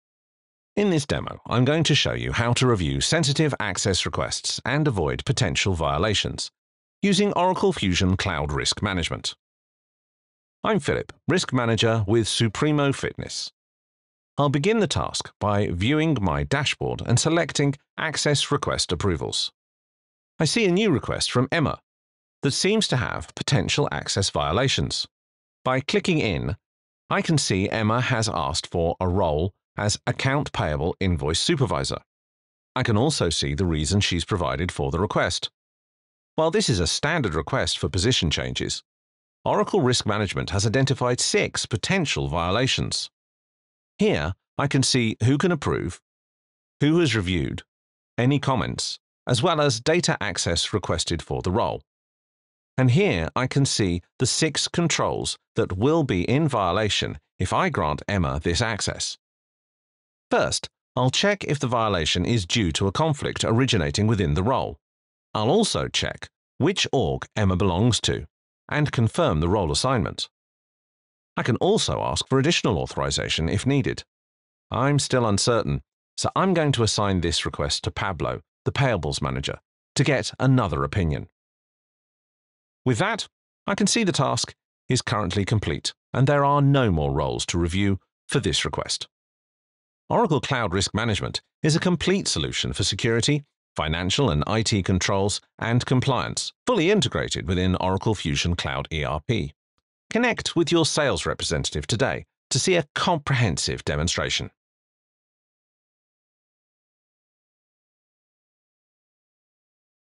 Avoid Violations with Oracle Risk Management: Demo
Explore how Oracle Cloud Risk Management, fully integrated with Oracle Fusion Cloud ERP, offers a complete solution for security, financial, and IT controls, as well as compliance. This video showcases how Oracle’s Cloud Risk Management Solution can enhance your Security Design and Separation of Duties processes. Watch now to learn more, and connect with your sales representative to schedule a comprehensive demo!